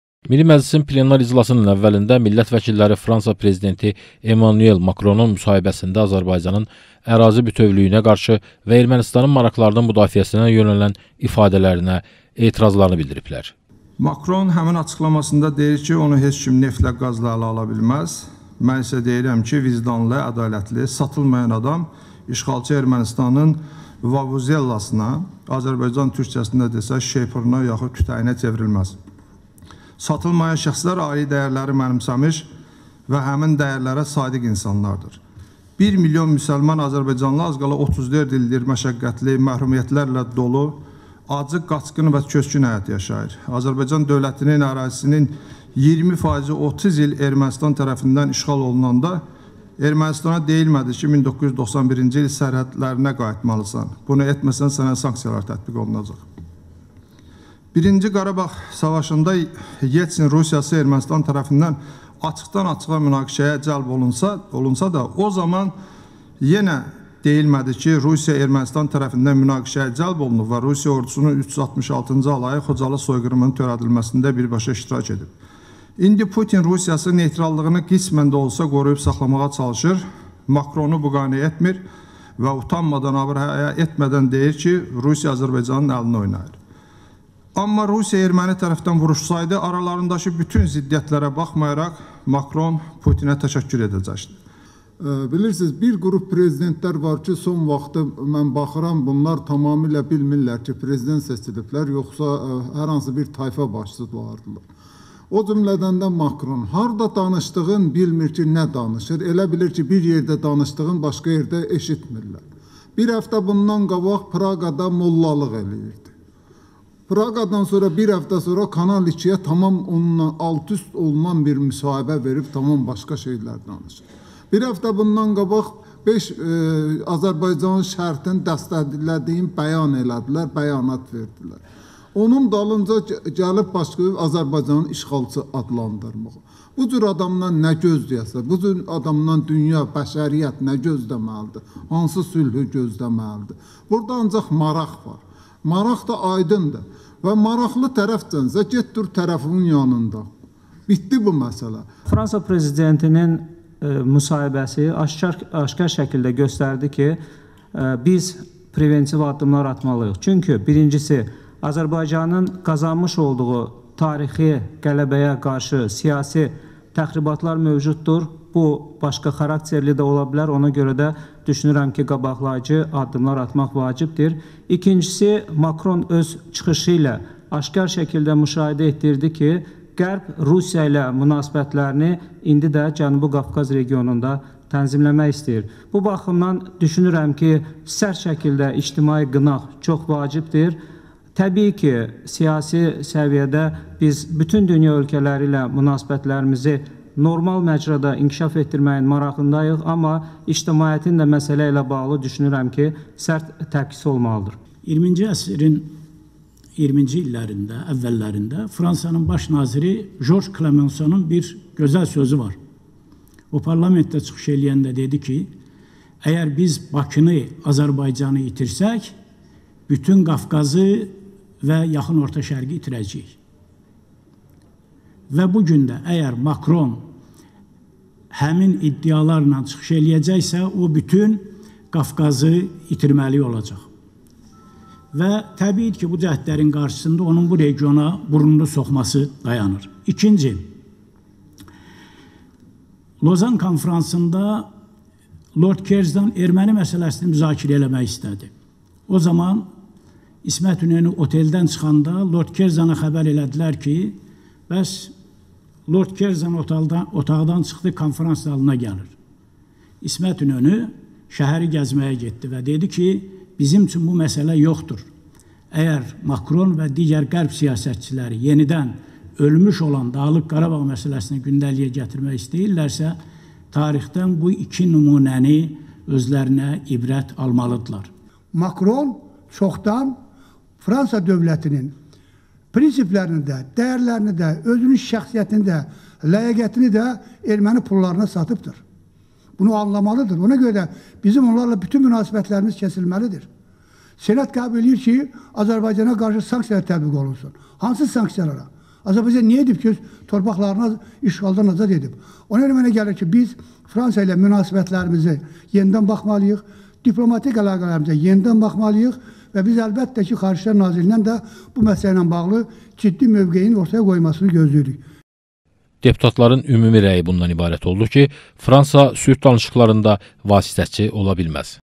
Milli Məclisdə Fransaya etiraz
Deputatlar Fransa ilə diplomatik əlaqələrə yenidən baxılmasını tələb etdi